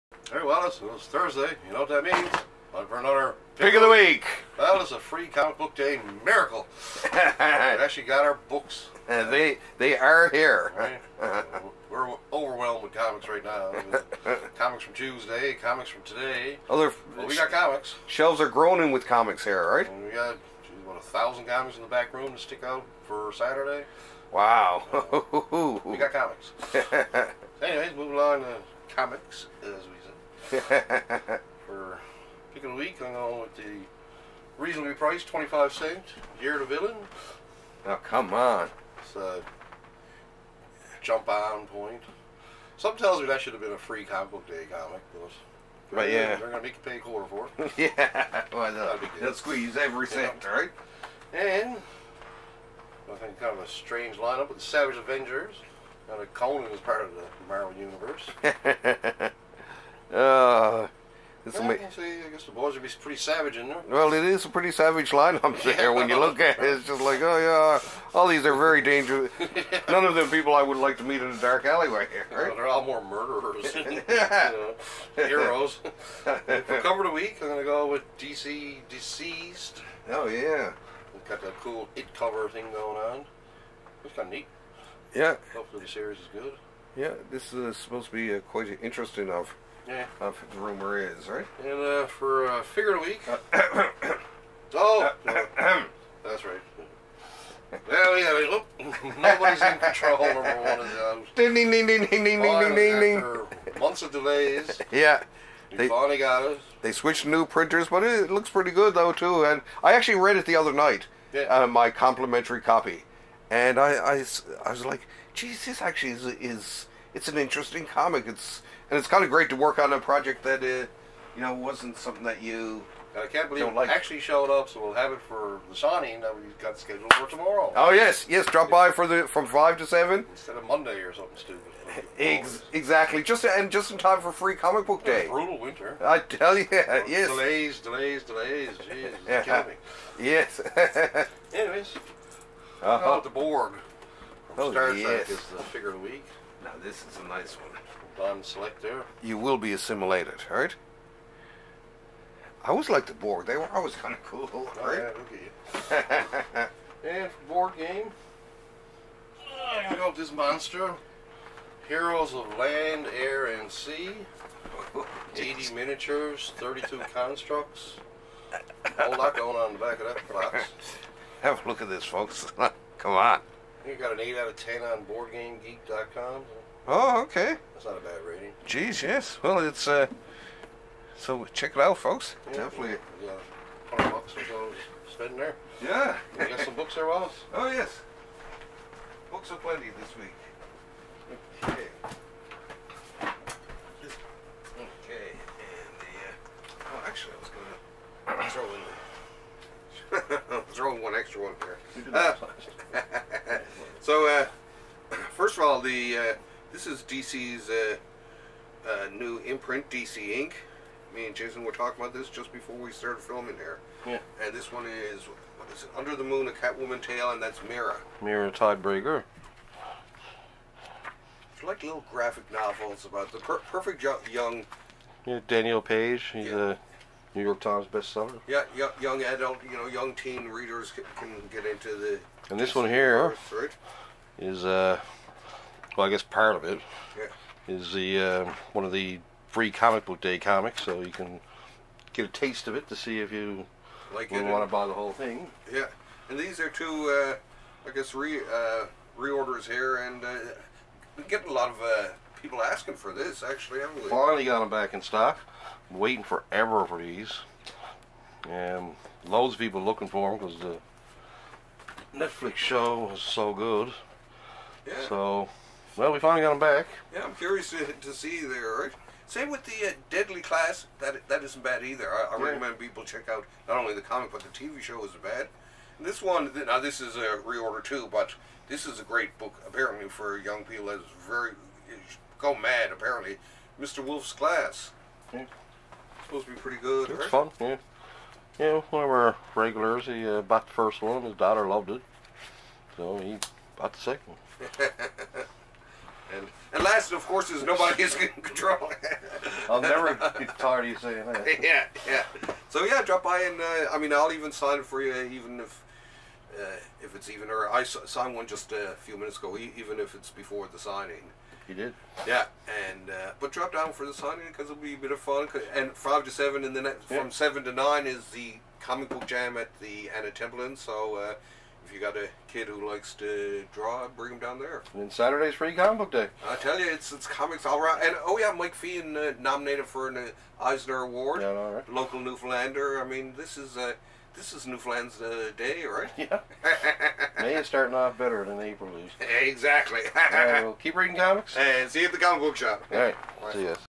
Downtown Comics Pick of the Week #103
Downtown Comics,

Starring Jason Conway & Wallace Ryan of Alphabet Comics!

The only thing better than A "Pick of the Week" is when one of the picks is something you worked on...in this case, the comic "Nobody is in Control" that artist Paul Tucker and I, in my role as Letterer, did for Black Mask Studios!!!

The reviews have been good so far and Midtown Comics made it one of their "Books of the Week"!!!

Special thanks to Jason for hosting our signing tomorrow from 5 to 7pm!!! A lot of the comic was actually lettered at Downtown Comics during our Cartoonist Nights!!! Drop by and get your copy signed!!!

More from the Headquarters for Comic Books in Newfoundland!!!

See you at the comic book shop!!!

A production of Wallace Ryan's Comics & Graphics!